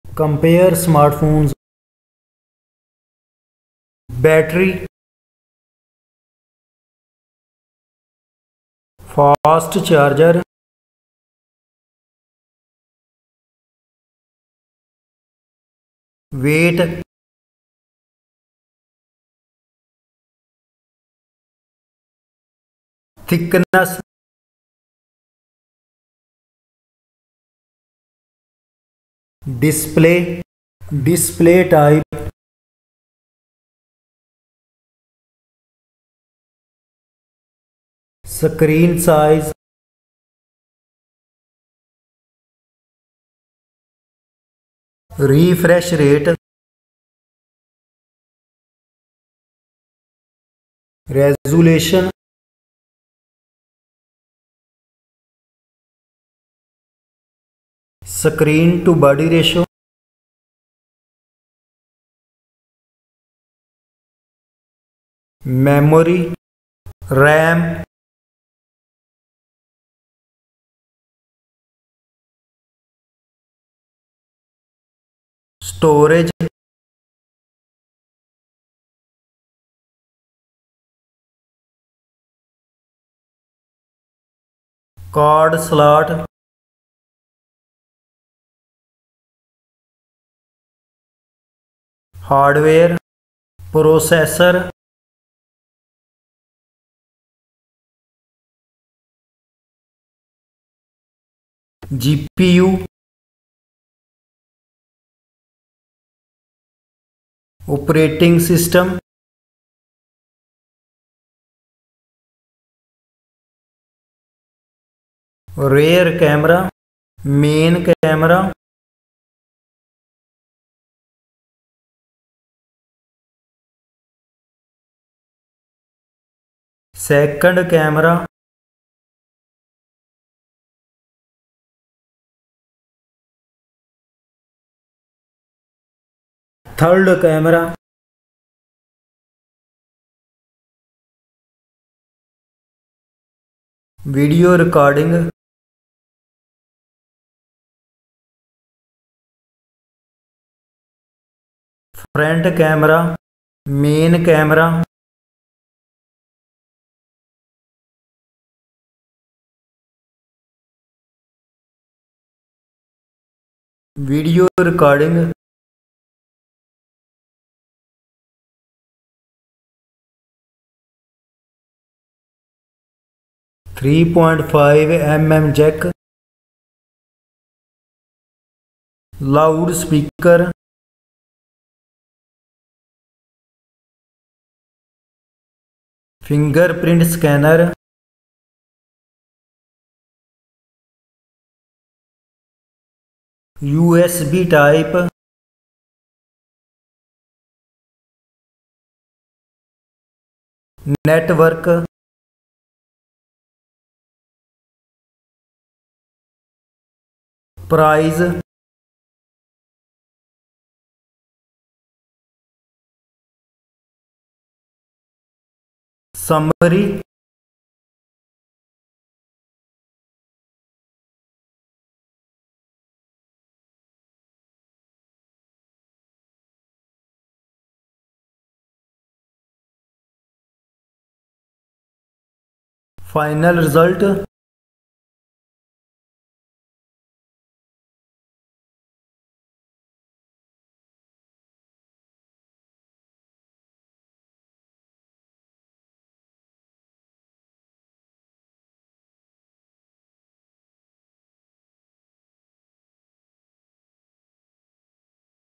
OnePlus Nord CE 3 vs Realme 11 Pro: Battle of the Mid-Range Titans!
🔥 Get ready for a showdown between two budget-friendly beasts! In this comparison video, we pit the sleek 📱 OnePlus Nord CE 3 against the power-packed Realme 11 Pro! 💪🏼 Which phone offers the best value for your money? 🤑 Join us as we dive into camera comparisons 📸, performance ⚙️, battery battles 🔋, and more! Whether you're a tech enthusiast or just hunting for a new smartphone, this is the ultimate guide to help you decide. 🎮📹

0:00 Intro
0:04 Battery
0:09 Fast Charger
0:16 Weight
0:23 Thickness
0:31 Display Type
0:38 Screen Size
0:45 Refresh Rate
0:51 Resolution
0:58 Screen to Body Ratio
1:07 RAM
1:15 Storage
1:25 Card Slot
1:33 Processor
1:42 GPU
1:47 Operating System
1:57 Rear Main Camera
2:07 Rear Second Camera
2:17 Rear Third Camera
2:24 Rear Camera Video Recording
2:33 Main Front Camera
2:44 Front Camera Video Recording
2:54 3.5 mm Jack
2:59 Loud Speaker
3:06 Fingerprint Scanner
3:14 USB Type
3:21 Network
3:28 Price
3:36 Summary
3:53 Final Result